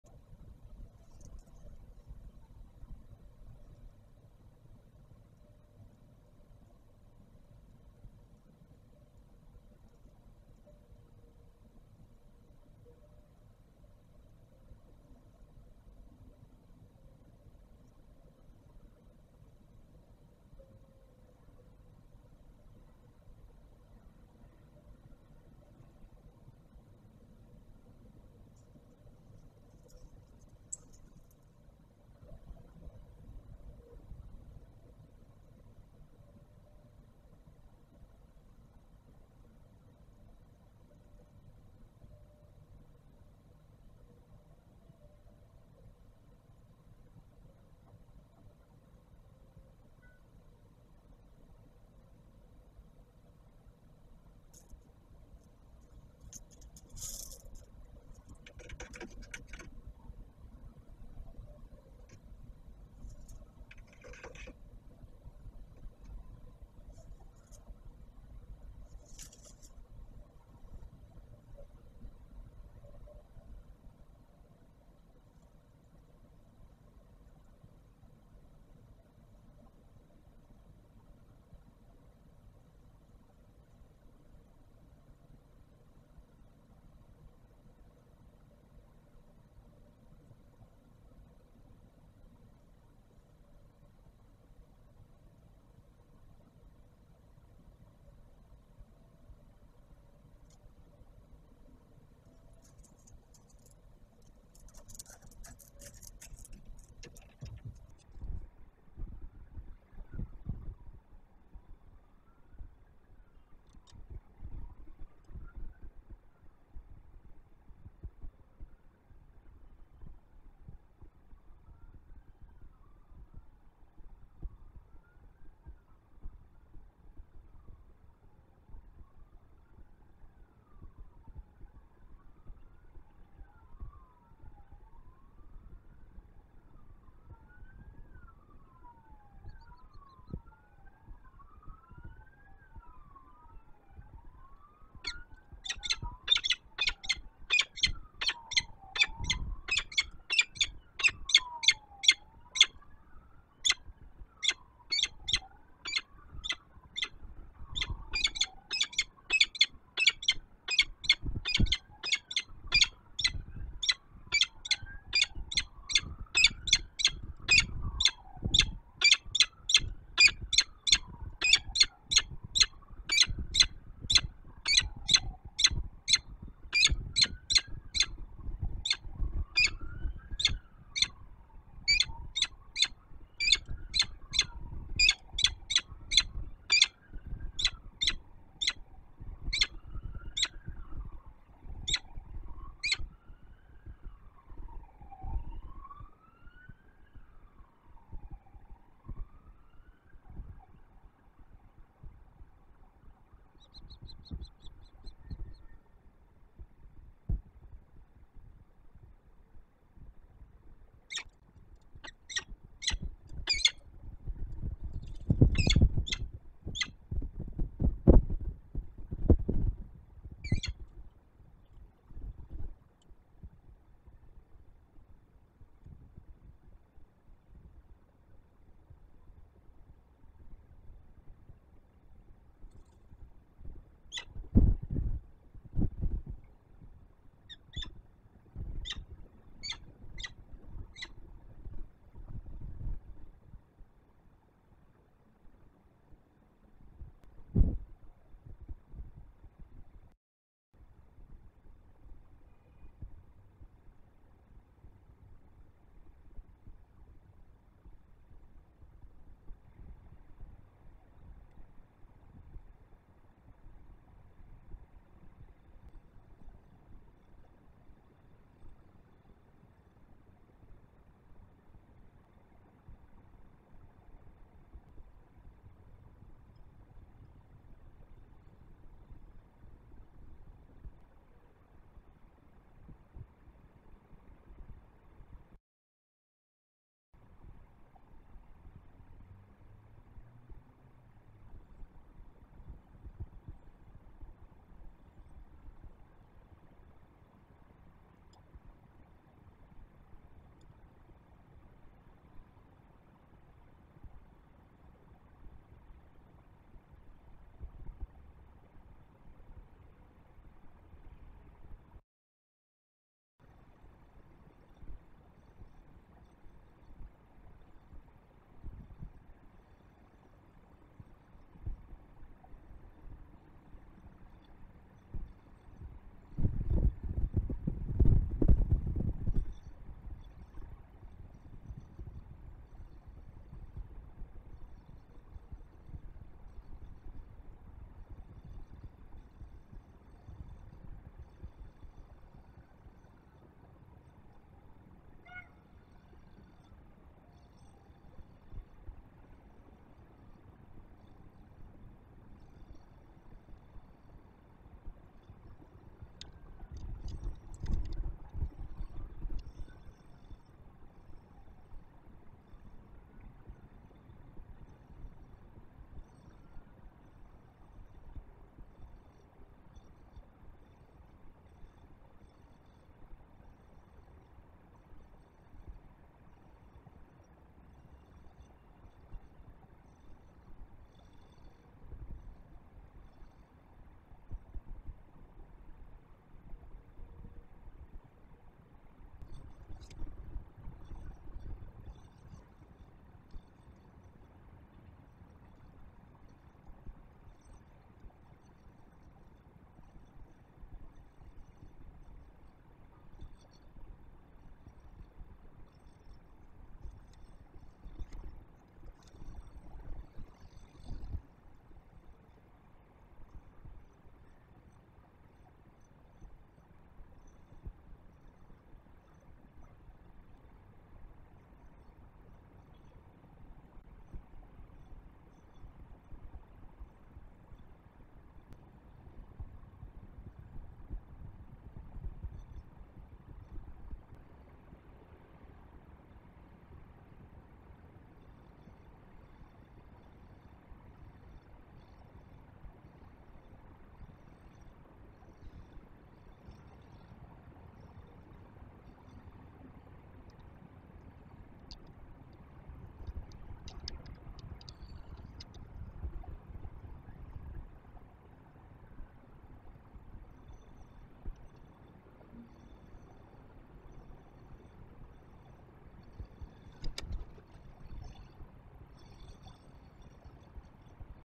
PKiN Warsaw PL - Giga in nest still, goes out to gallery. Someone flies over her-2024 09 19 06 54
She gives a voice, sits on the threshold and flies away.